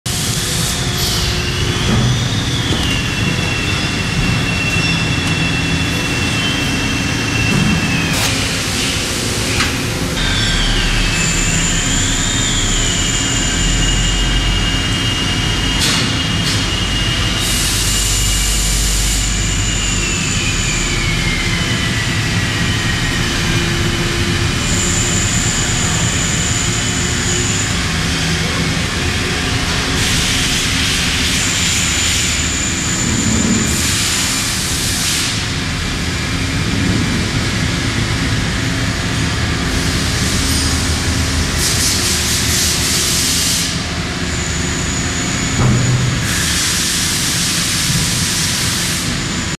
1996 Monarch Predator CNC Lathe- Fanuc 18-T - 2 axis - 8" Chuck - 30VDI turret
1996 Monarch Predator CNC Lathe
Fanuc 18-T Control- 2 axis chucker - 8" chuck - 12 Station VDI Turret
200 x 600
200 swing
8400 LBS